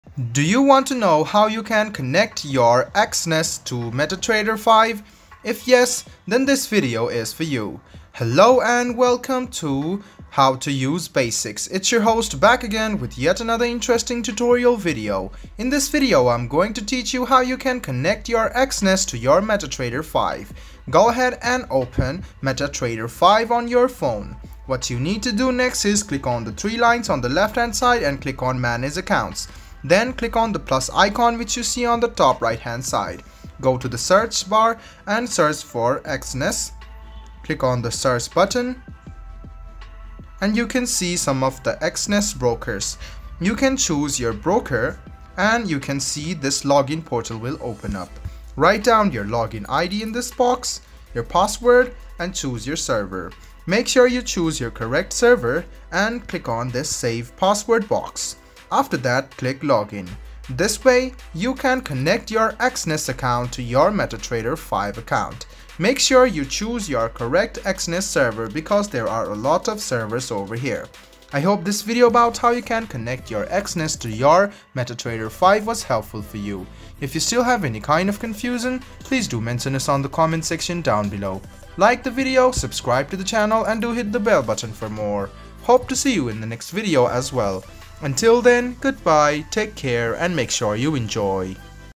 How to Connect Exness to Metatrader 5 in 2024 | Metatrader 5 Tutorial
Watch this tutorial to learn how to connect your Exness account to MetaTrader 5. Follow our comprehensive instructions to integrate your Exness trading account with the MT5 platform, allowing for a smooth trading experience.